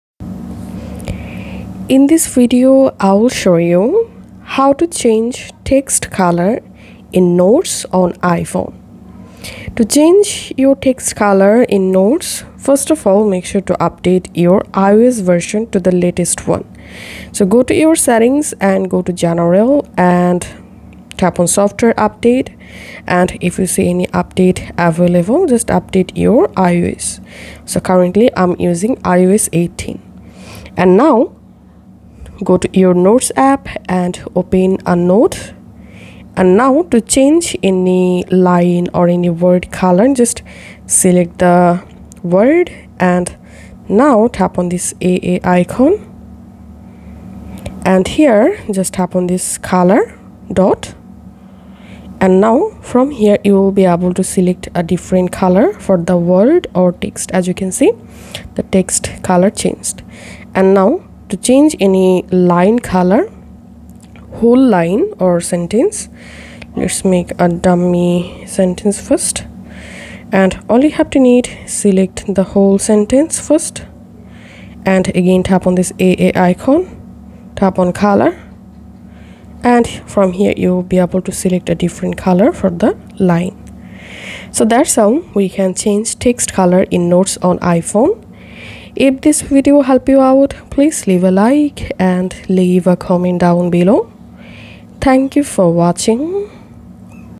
iOS 18: How to Change Text Color in Notes on iPhone
Unable to change the font color in the Apple Notes app? Here are two simple ways to change the font color in iPhone and iPad notes.